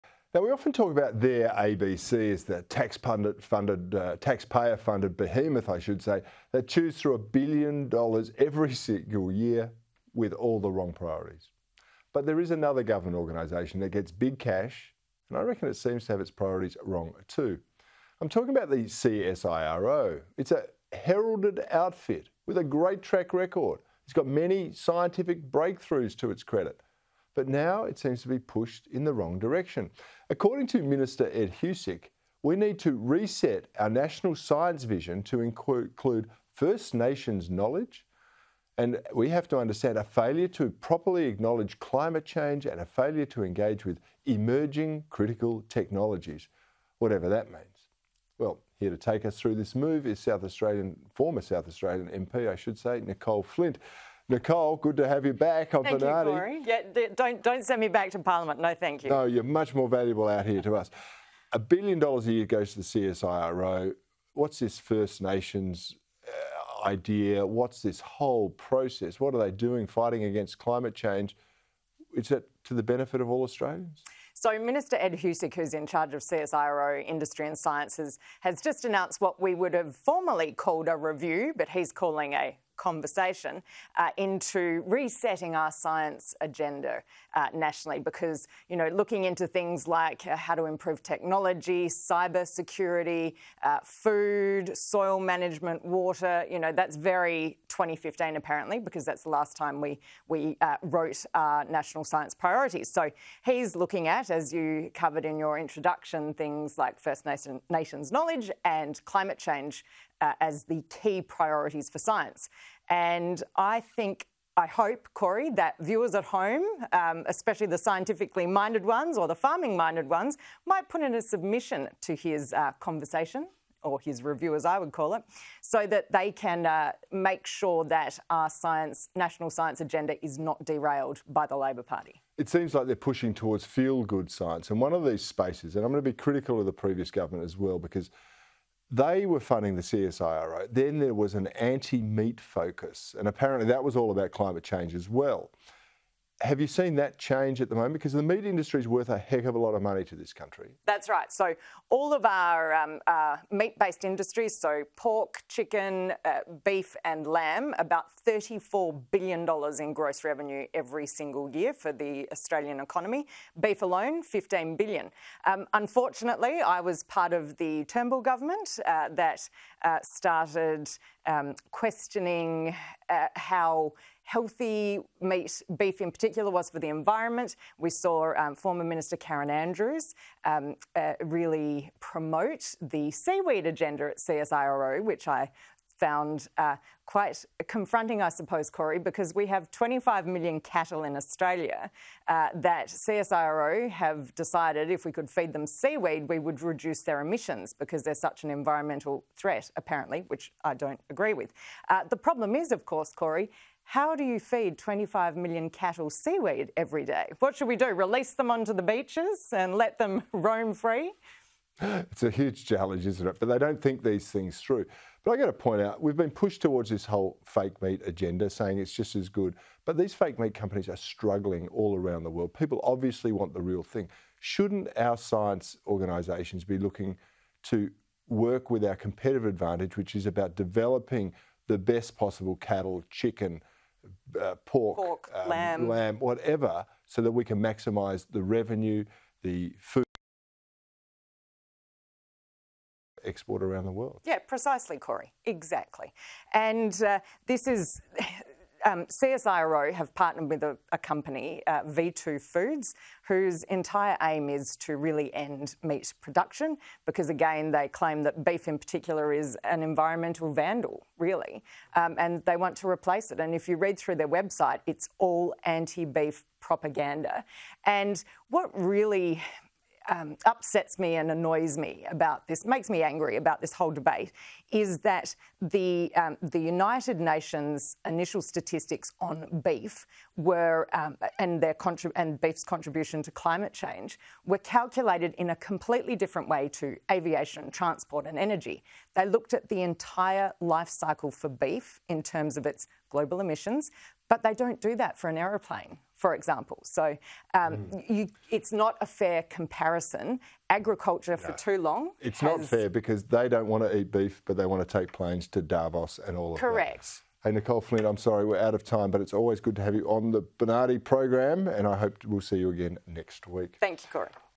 CSIRO pursuing ‘feel-good’ science
Sky News host Cory Bernardi says the CSIRO is pushing towards “feel-good” science.

Mr Bernardi said there was an “anti-meat” focus at the organisation when the Coalition was in government.

“Apparently that was all about climate change as well,” he said.

Mr Bernardi discussed the issue with former Liberal MP Nicolle Flint.

“CSIRO have partnered with a company, v2food, whose entire aim is to really end meat production,” Ms Flint said.